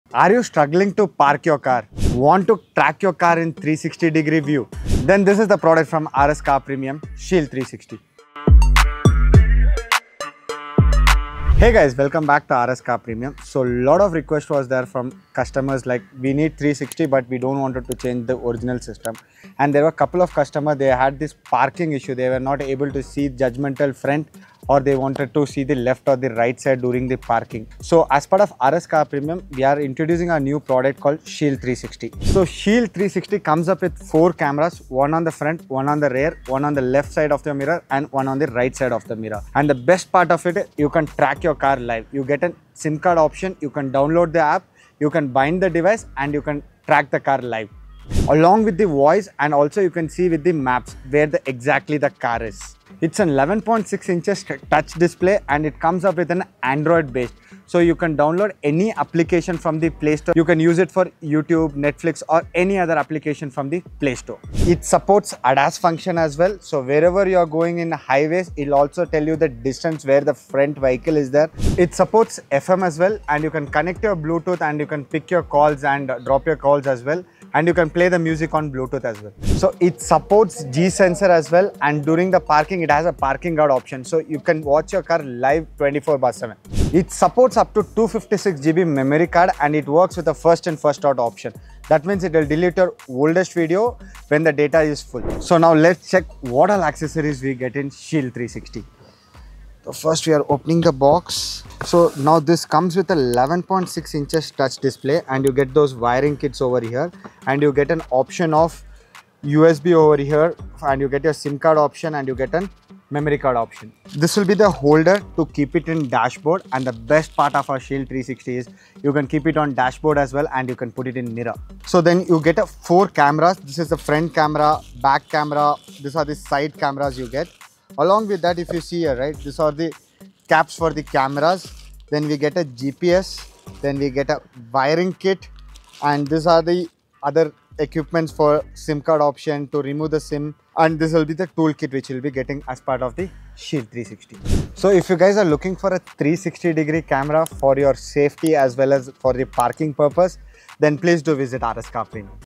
Shield 360 | 360 degree DashCam for your car |Best Dashcam
Rscarpremium  Shield 360
Android 9.0 os,  4CHS video recording, GPS navigation & track, WiFi hotspot , 4G SIM card  and Remote Monitor ect Multiple functions , All in One  Vehicle Fleet Management Mirror DVR  and Dashboard installation car dvr. CarAssist app and CMSV6 platform are available for remote monitor and vehicle fleet monitor.

Book your entertainment system now.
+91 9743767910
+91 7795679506